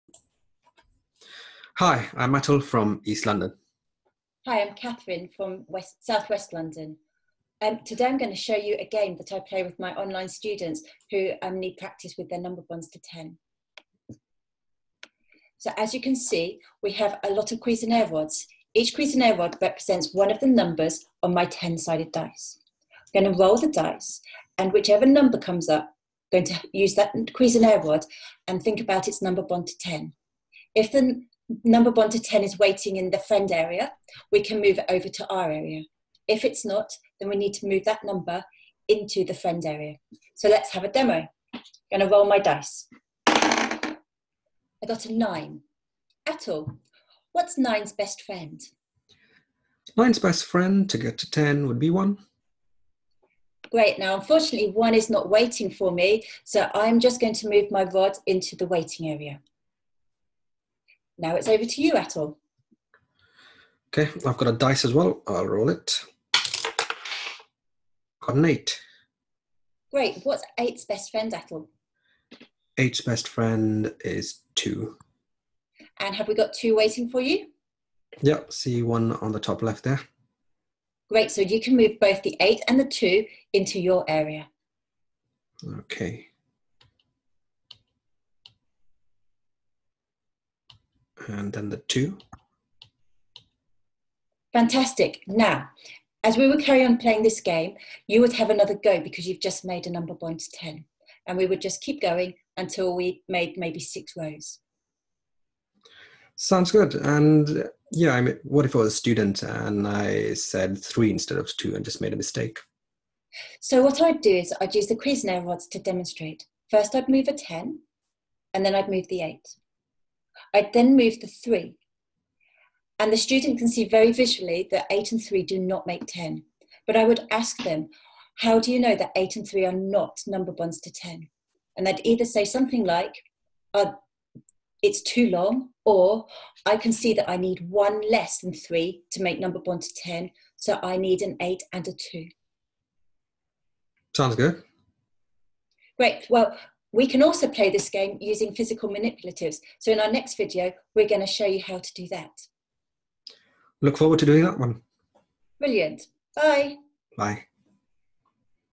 Number bond game with Virtual Cuisenaire Rods - Online Tutoring Demo
Catherine Rooke does a live online maths tutoring demo to Atul Rana. The demo shows a number bonds game that can be played with students using virtual cuiesnaire rod manipulatives and a ten sided dice. The rods can be moved around on a shared and interactive digital paper board called BitPaper. Note that the original game was introduced to us by Ronit Bird, Dyscalculia specialist.